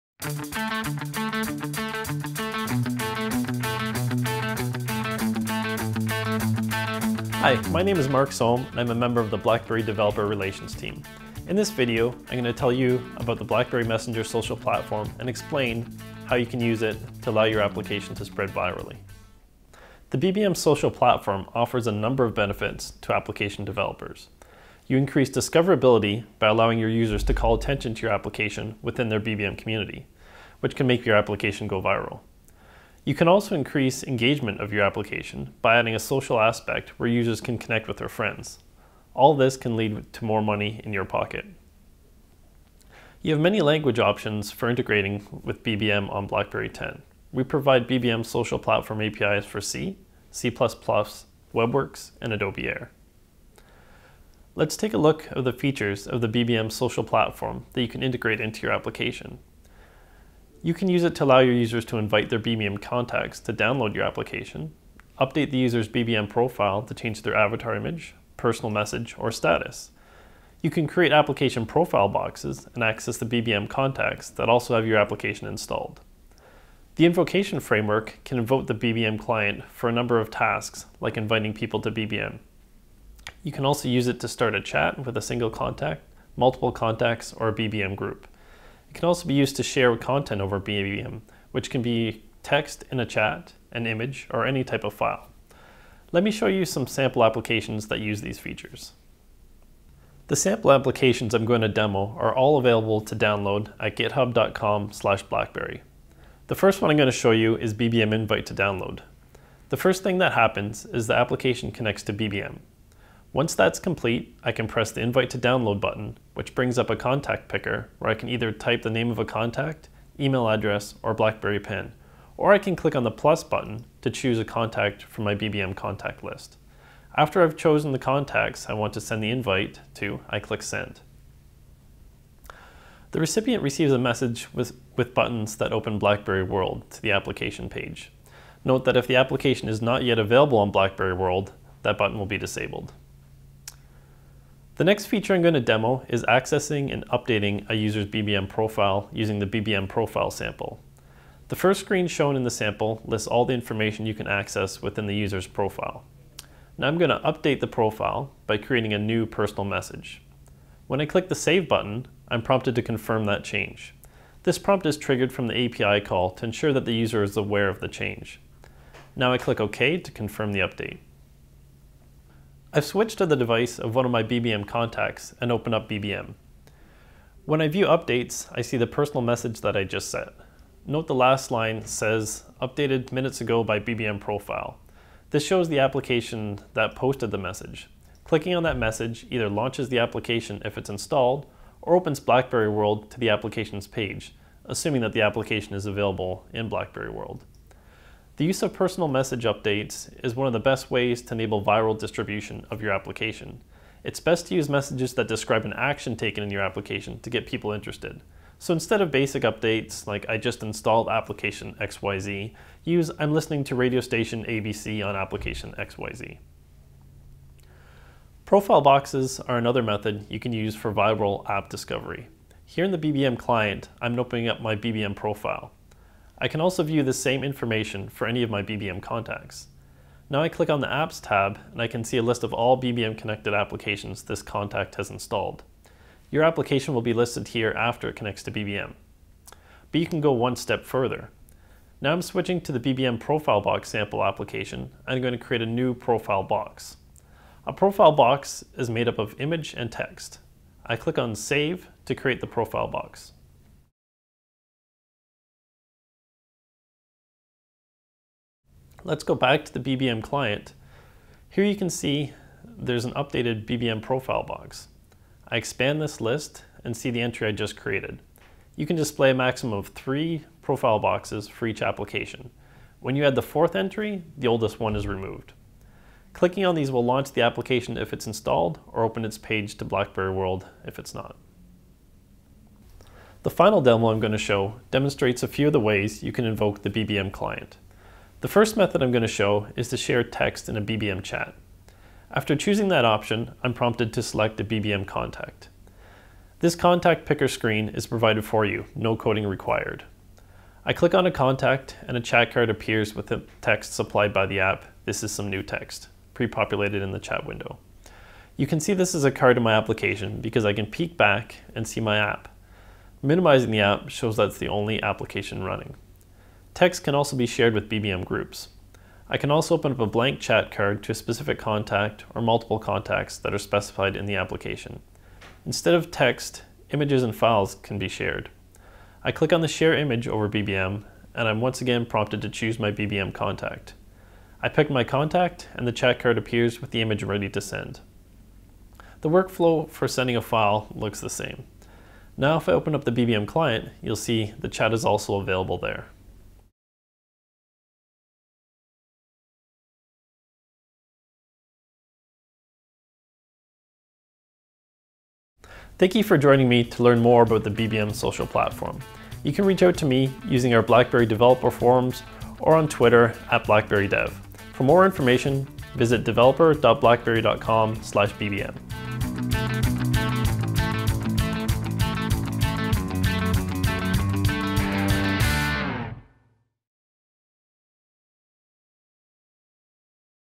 BBM Social Platform
Meet the BlackBerry® Messenger Social Platform (BBM™ SP), which you can use to enable your application to go viral with very little effort.  Watch this video for a high level overview of what can be done using the BBM SP APIs.